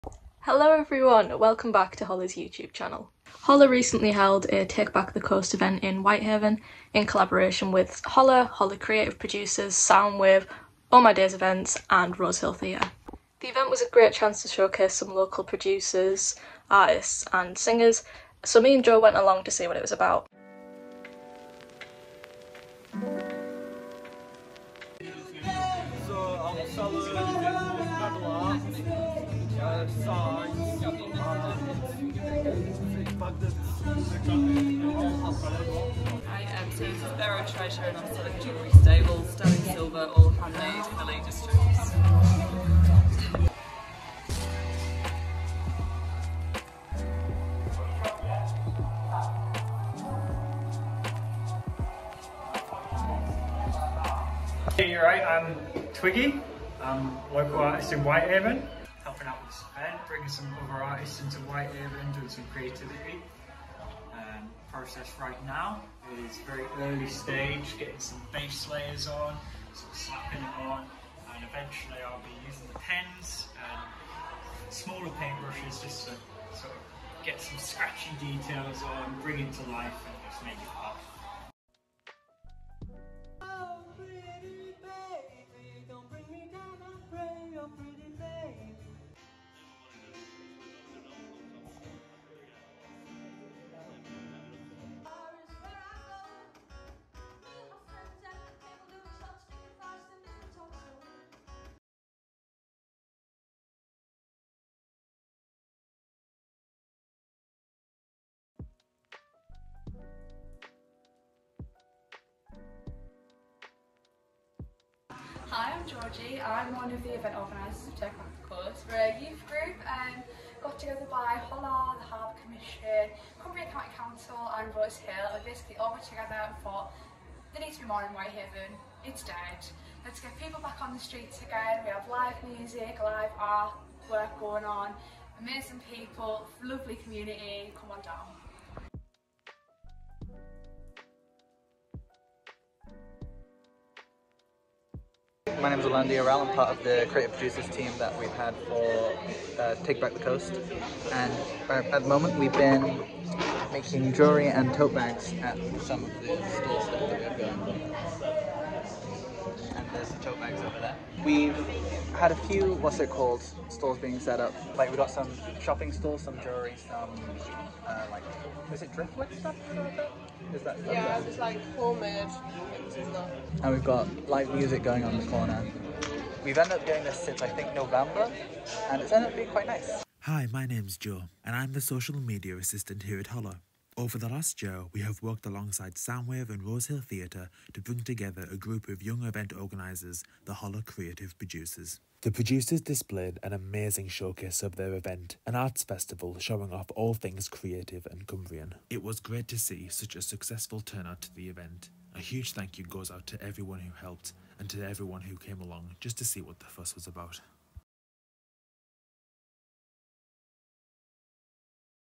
HollrEXPLORES: Take Back the Coast
Hollr welcomes the TAKE BACK THE COAST event to the Rum Story, Whitehaven, in collaboration with:
- Soundwave
- Hollr Creative Producers
- Rosehill Theatre
- Cumbria County Council

Find all the amazing creatives featured in this video on Instagram, and us @hollr.site